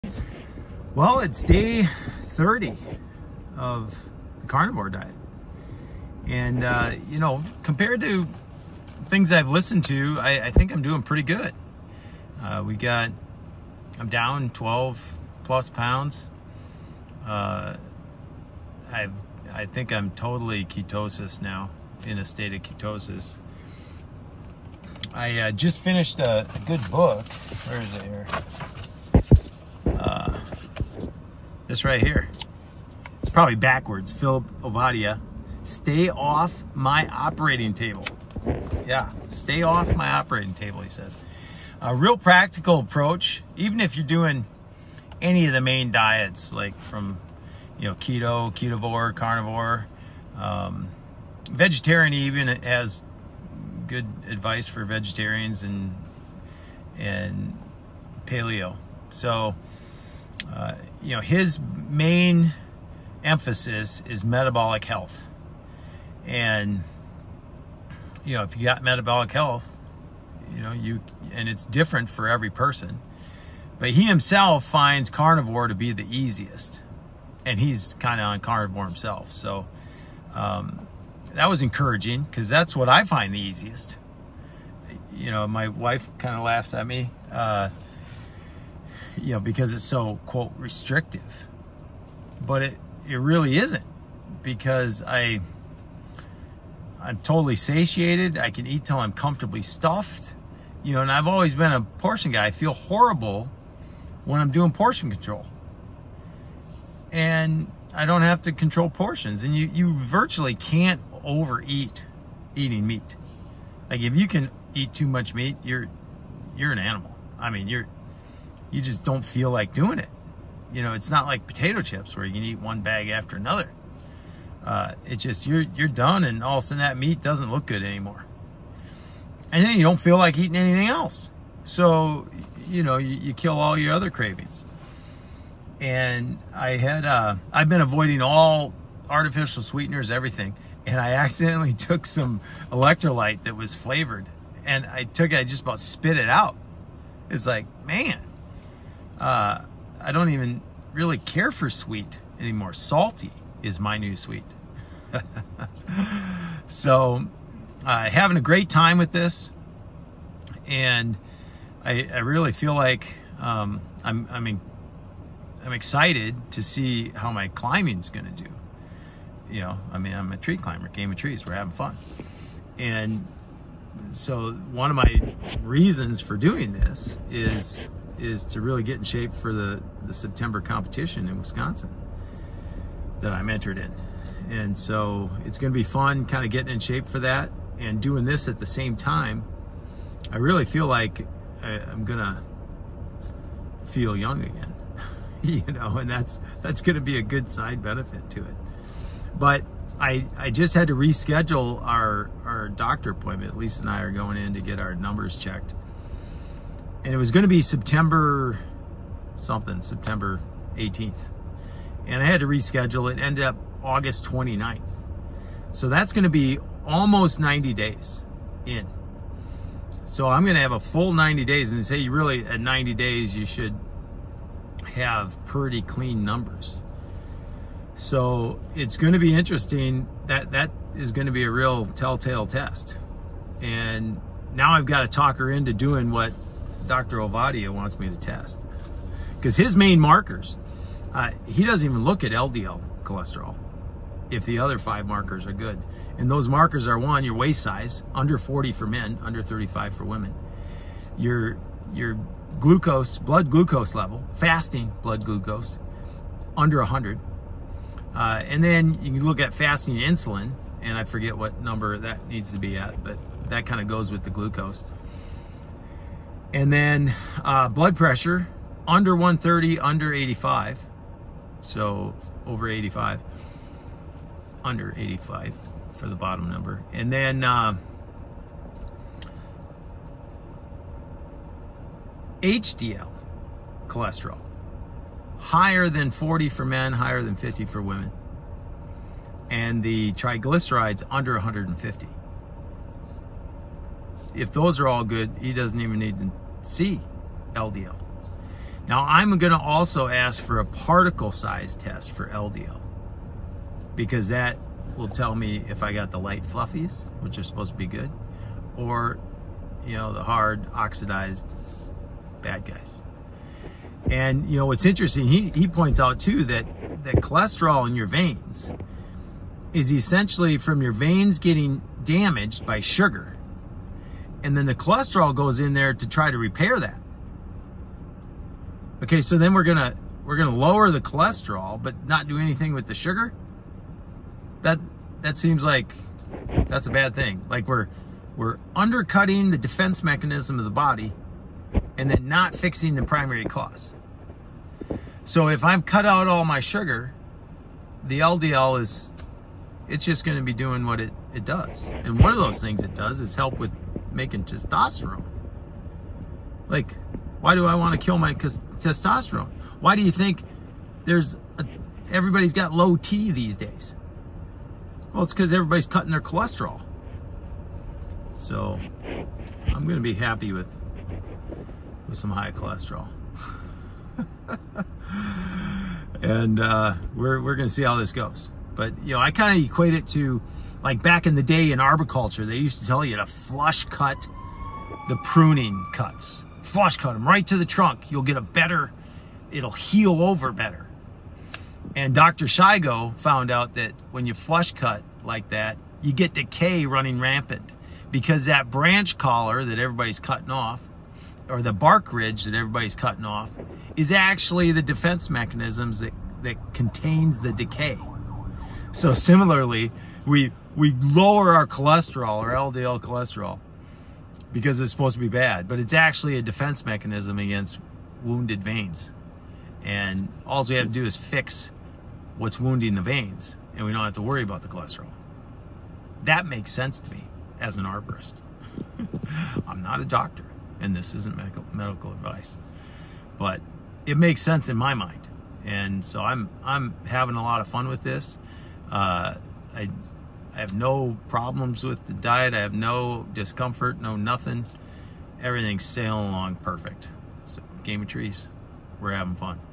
Carnivore Diet Day 30
I’ve listen to a number of people talking about day 30 and I think my day 30 is better than a lot of people’s day 30. I’m feeling great and highly motivated. This just might be my lifestyle. I finished, “St￼ay Off My Operating Table”, by Philip Olvadia MD. I highly recommend his book if you’ve ever thought about changing your diet. He has a great approach that focuses on overall metabolic health.